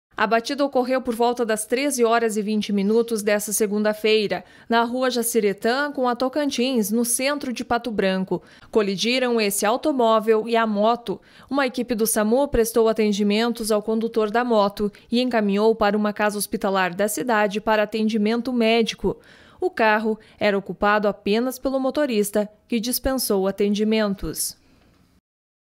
Condutor de moto fica ferido ao se envolver em acidente em Pato Branco
Segurança: Em Pato Branco, acidente envolve carro e moto. Uma pessoa fica ferida.